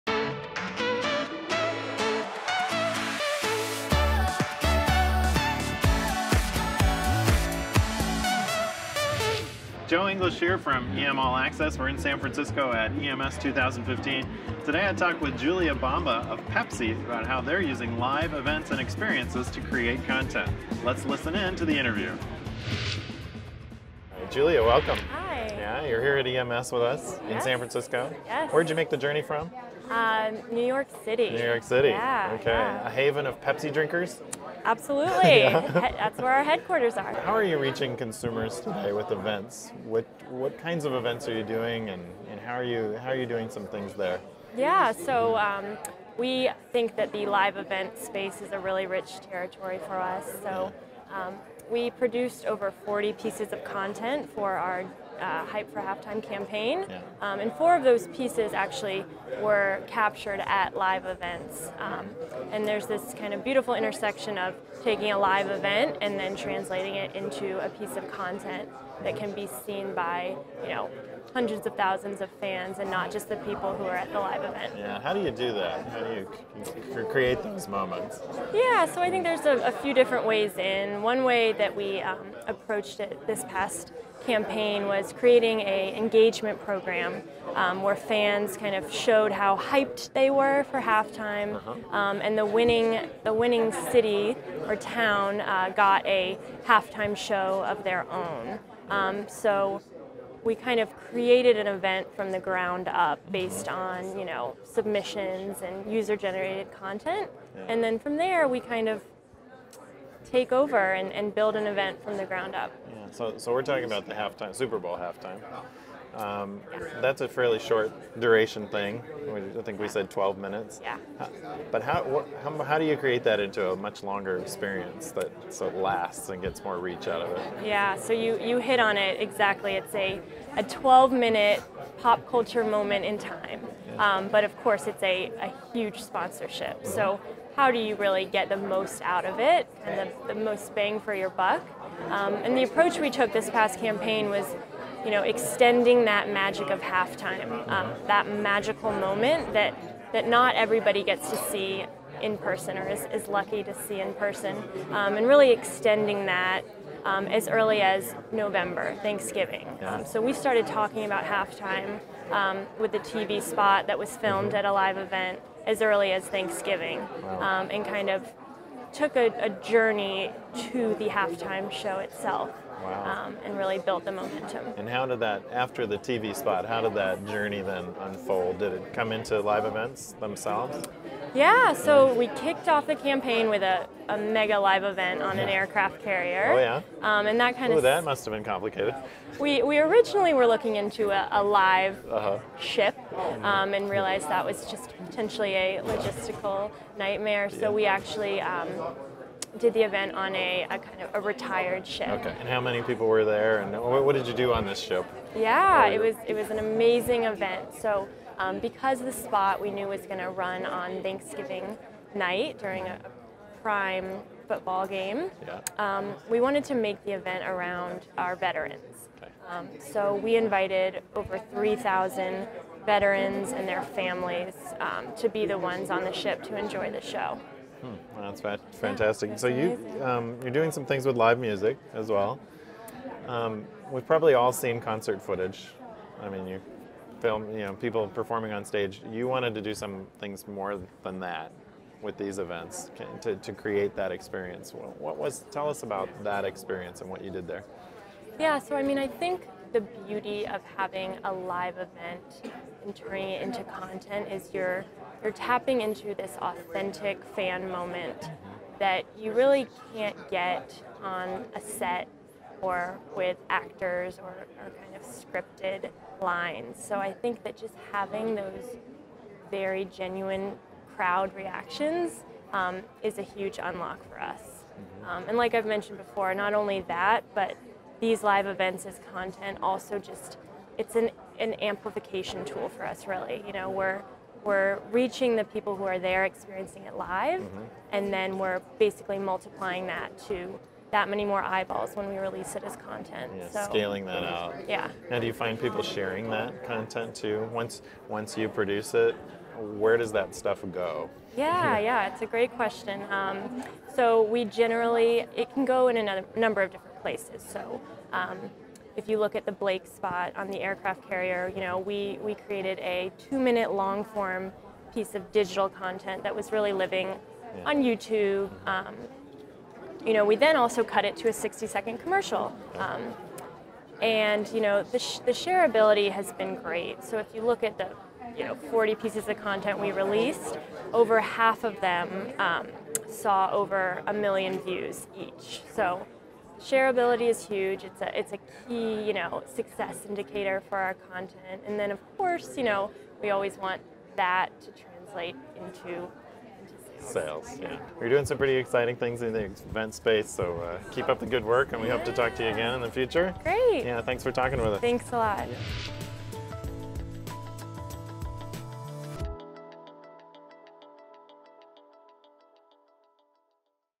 EM All Access: Pepsi's Tips for Using Live Events as Content Generators
In this episode of EM All Access, Pepsi Brand Manager Julia Bomba talks about how the brand uses live events as content generators. Hosted by Event Futurist Joe English.